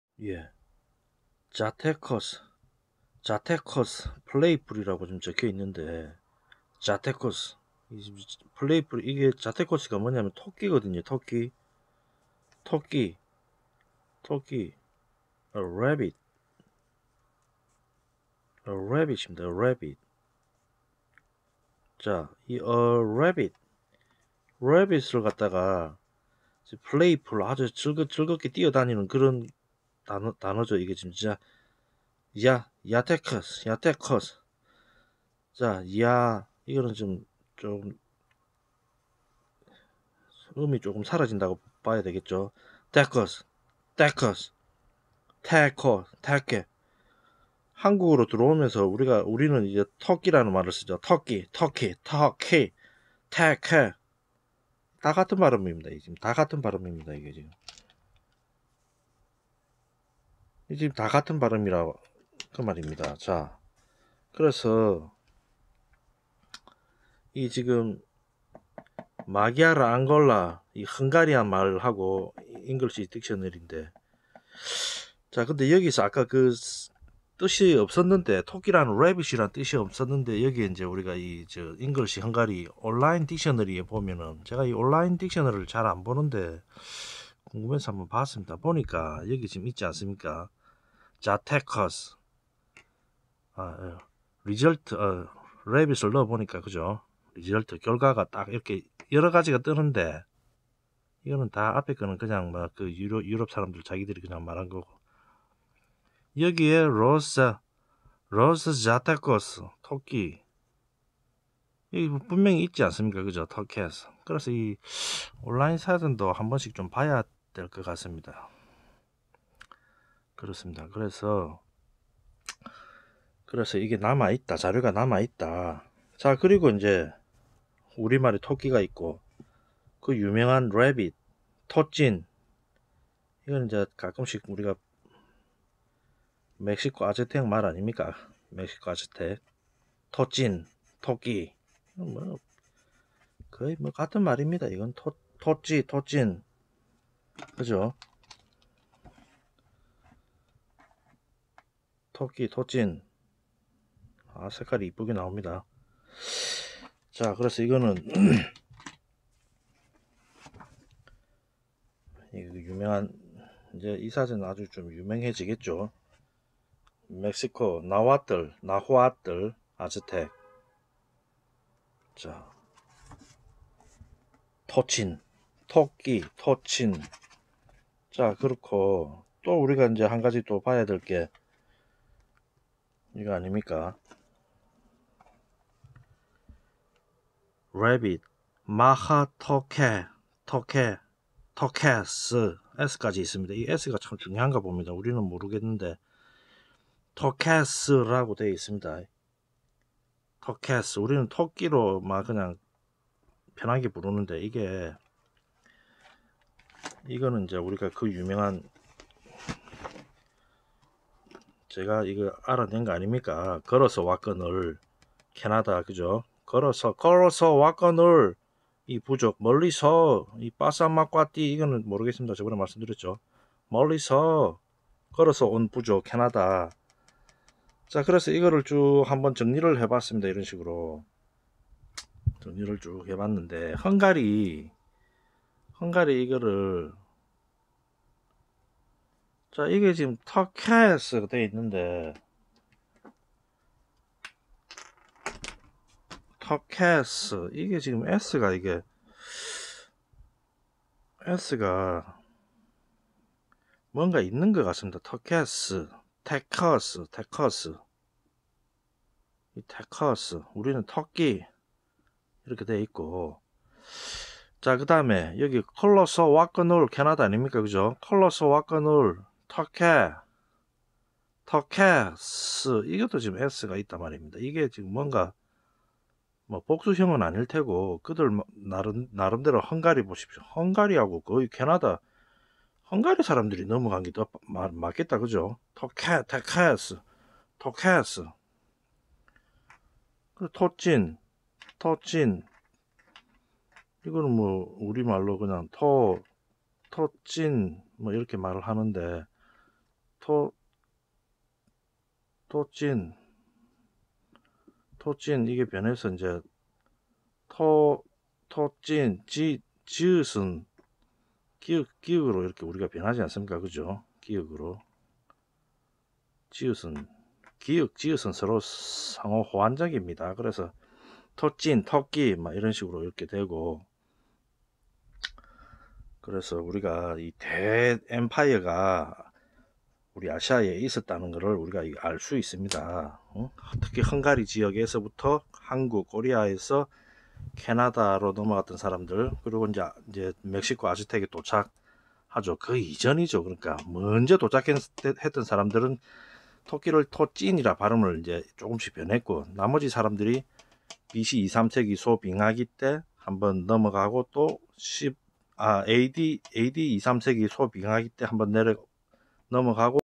81) 토끼 rabbit : Hungary, Canada 원주민, Aztec , '토끼' 단어는 동북아시아의 표준어였다.
'토끼'라는 단어를 동유럽에서 멕시코 아즈텍까지
광범위한 지역에서 말했음을 확인할 수 있다.
제국이 존재했었다고 말하고 싶지만 제국다운 건물이
동유럽이나 고대한국땅에 남아있지 않아 아쉽다.
동아시아에는 아즈텍 신전의 위용에 버금가는 건축물이 존재했었고
이런저런 이유로 파괴되었다고 말할 수 있겠다.

*헝가리 : '집토끼' 비슷하게 발음하는 점이 인상적이다.
*'걸어서왔거늘' 캐나다 원주민: togehs .
 (부족 명칭이 Kolusu Wakonol '걸어서 왔거늘'임에 또 한번 놀라고
ㅡ  실수일 수도 있습니다.  한국에 아메리카 원주민 학과가 생기는 그 날까지 )

*멕시코 아즈텍 : 손성태 배제대학교 교수 때문에 유명해진 아즈텍 신전 방문 영상에 소개된 tochin.